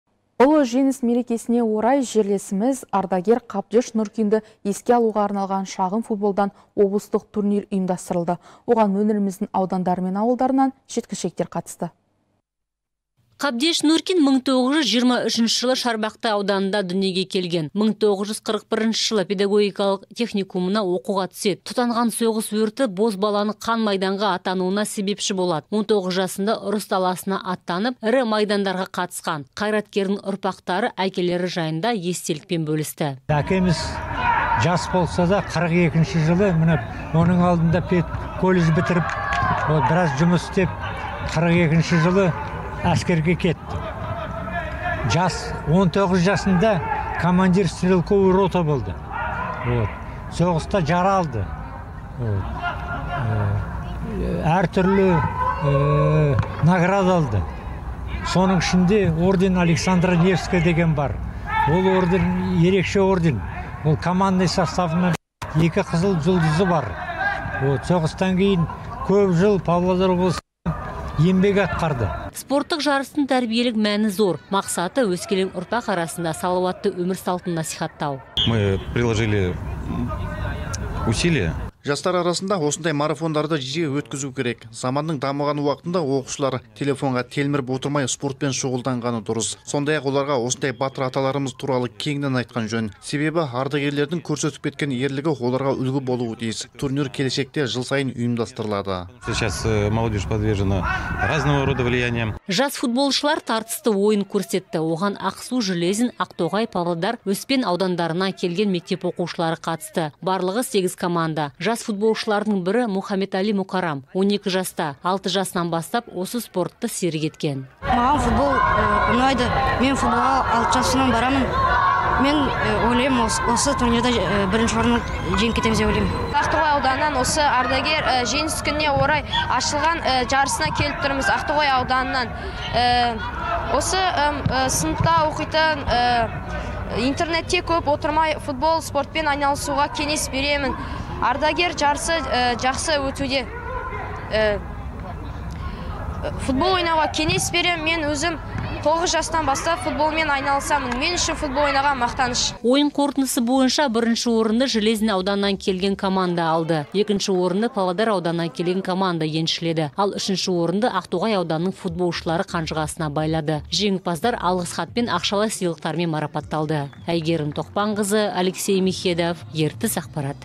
Ардагерді еске алуға арналған турнир өтті
Ұлы Жеңіс мерекесіне орай жерлесіміз, ардагер Қабдеш Нұркинді еске алуға арналған шағын футболдан облыстық турнир ұйымдастырылды. Оған өңіріміздің аудандары мен ауылдарынан жеткіншектер қатысты.